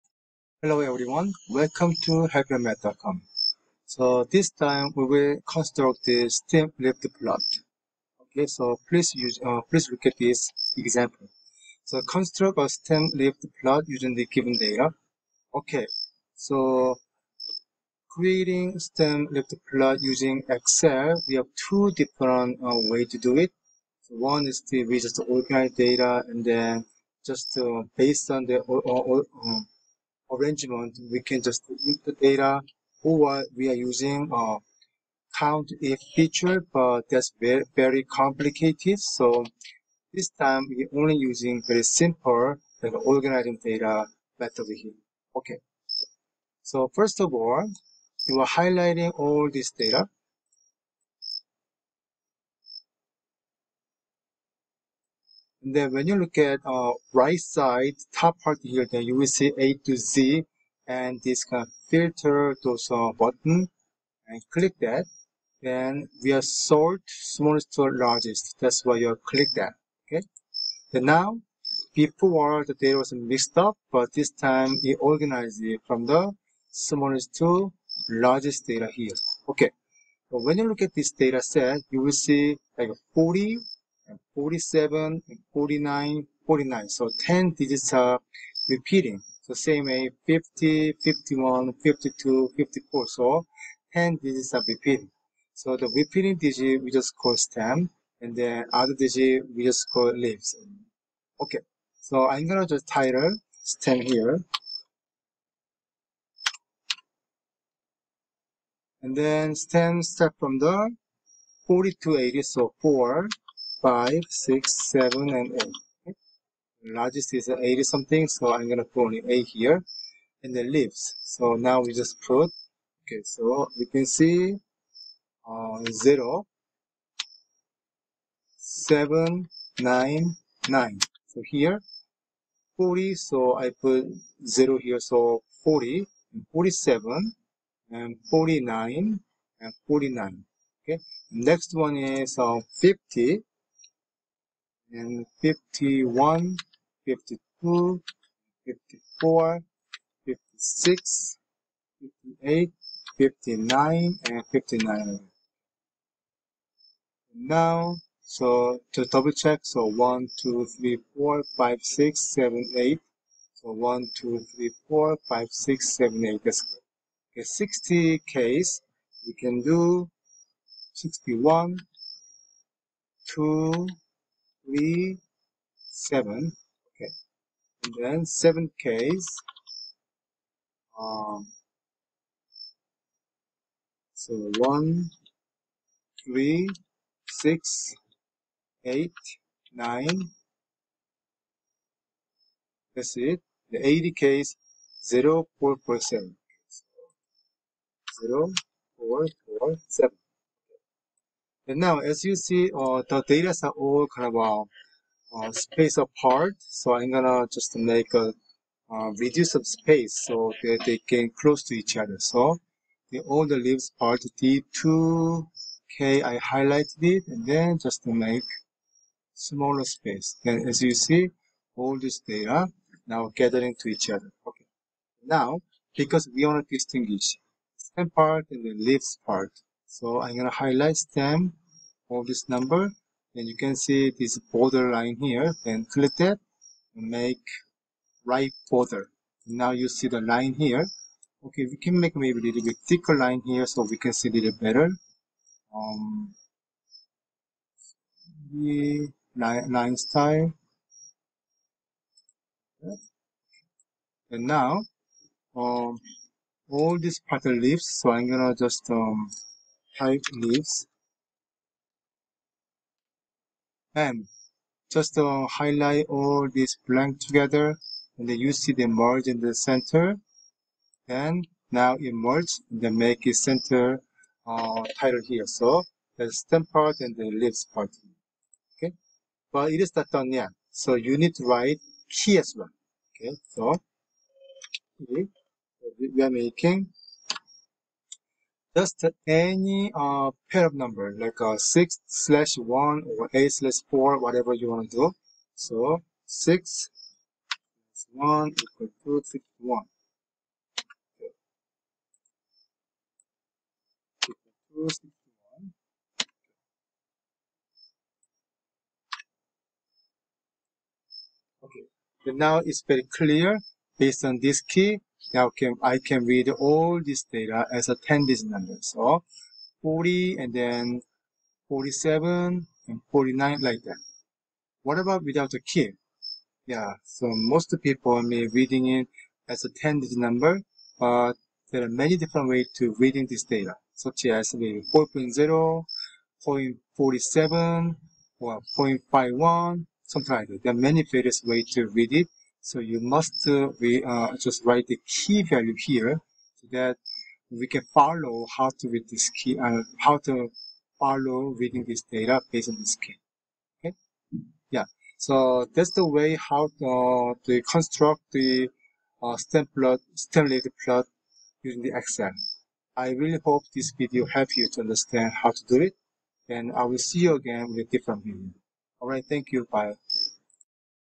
MAT 209 Lesson 3.1 Stem Leaf plot using Excel
This video demonstrates how to construct a Stem-Leaf plot using Excel.